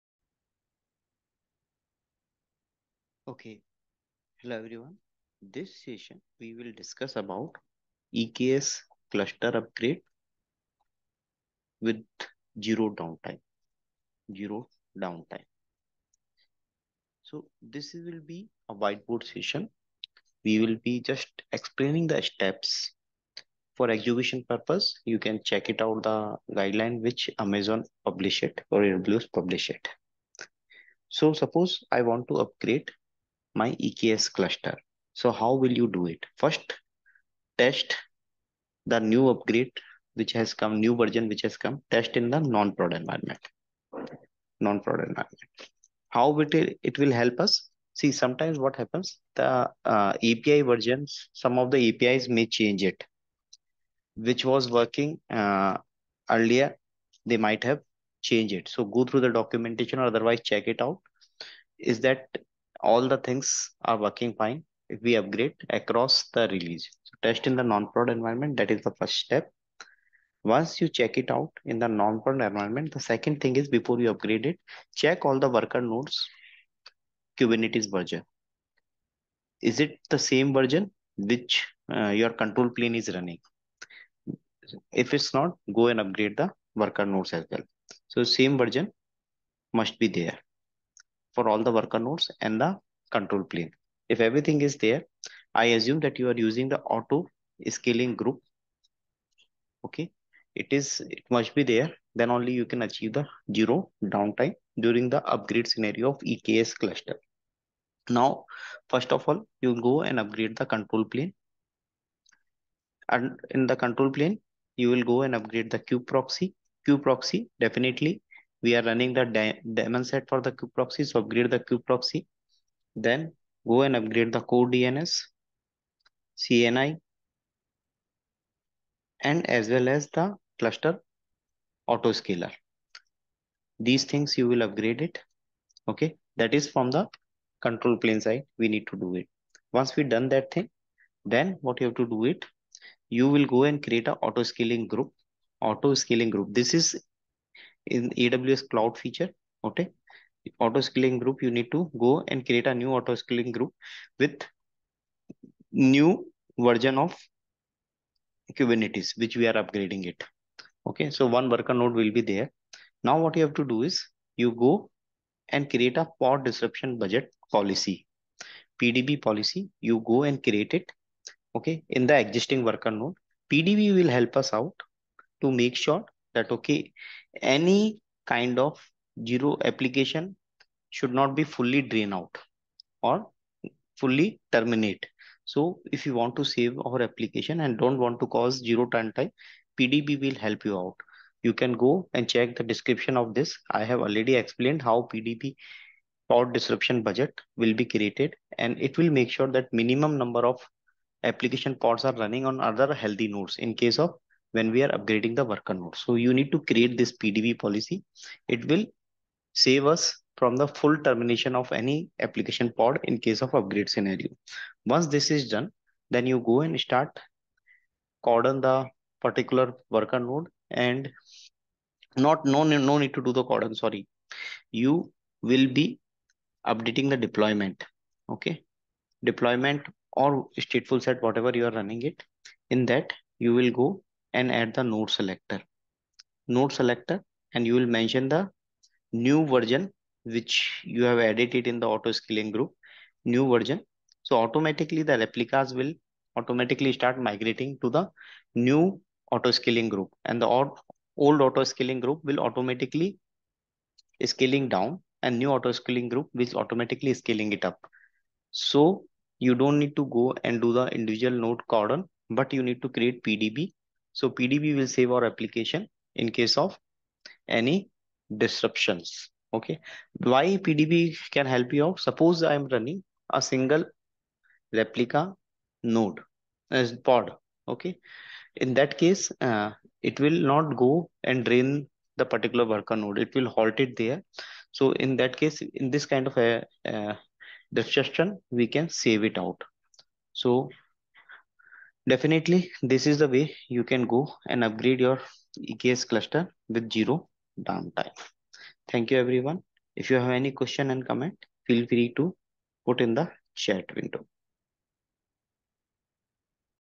EKS cluster upgrade (Explanation): Zero downtime
This is whiteboard explanation about the steps how we can upgrade eks cluster with zero downtime